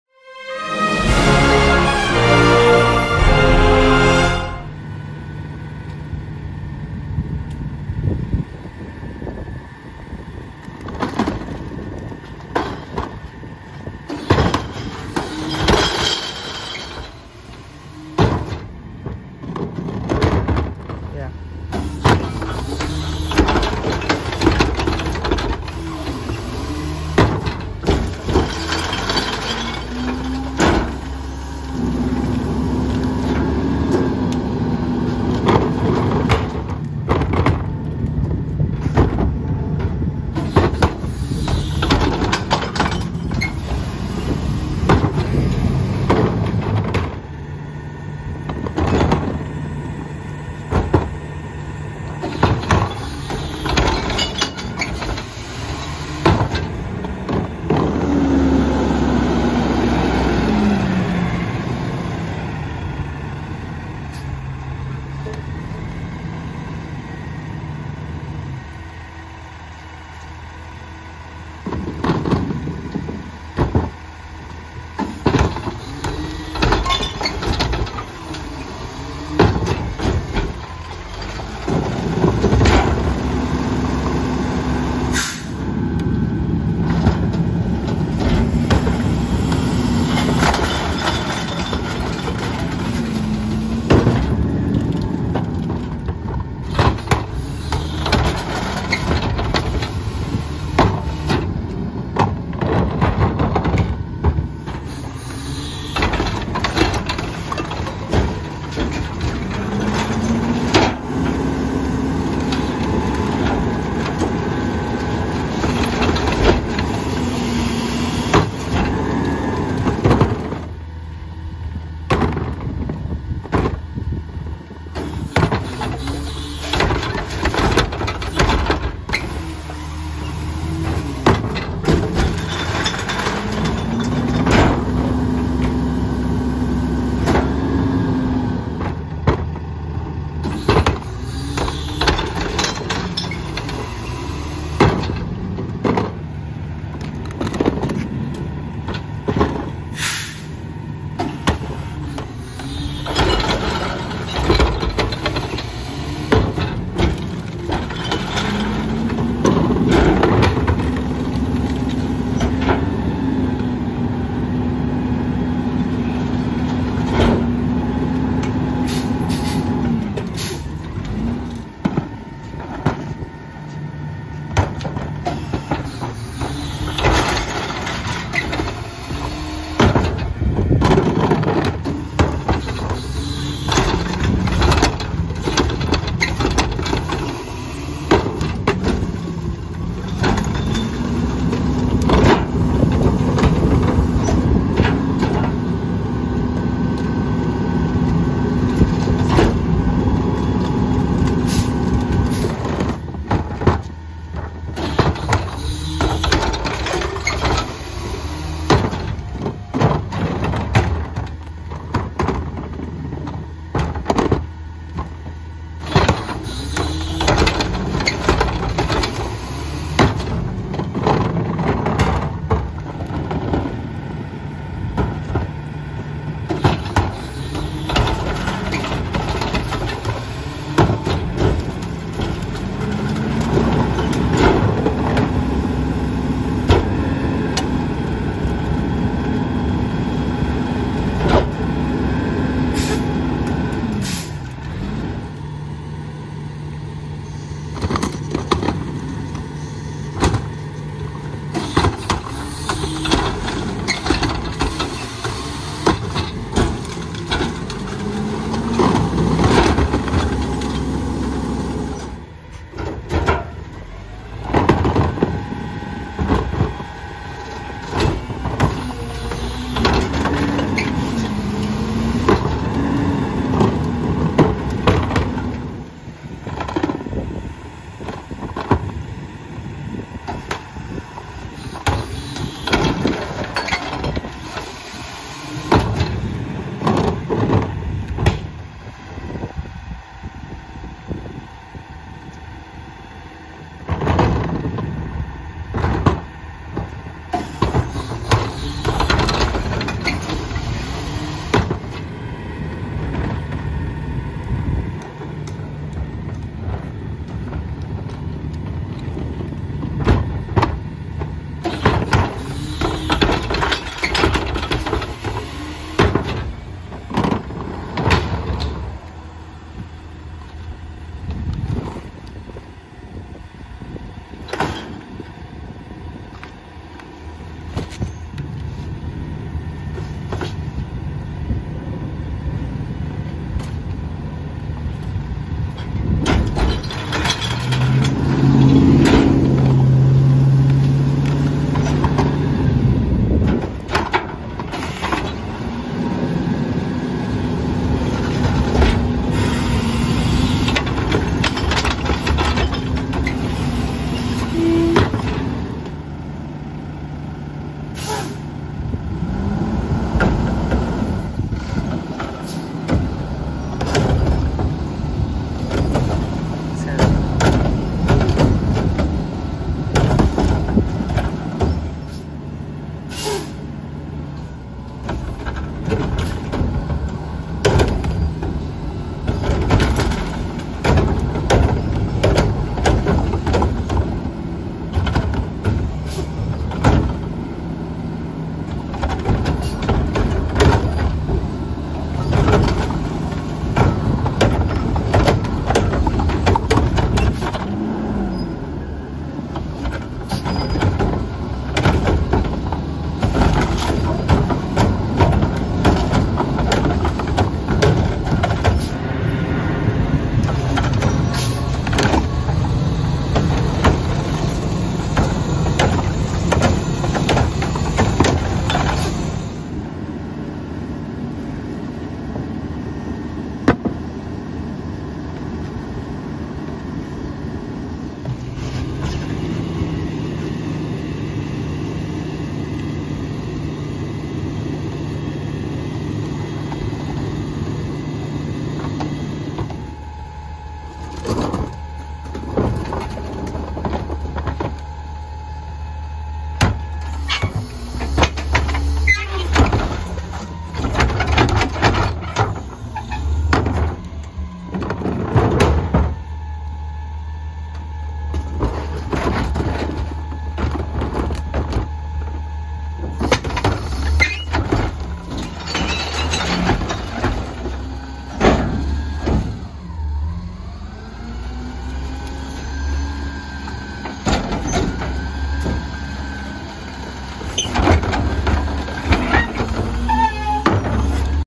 WM Mack LR Rear Loader Garbage Truck Packing Carted Recycling
Here is something I did not expect to find in Elk Grove, a mcneilus rear loader during recycling. wm almost never has rear loaders on residental with exceptions on yard waste and alleys so this was really cool to film. Also pretty interesting to see an LR rear loader since WM around here hasn’t been buying a ton of them, going frontload pretty much everywhere. The speed on the Tipper was cool to see too

Thanks to the driver for letting me film!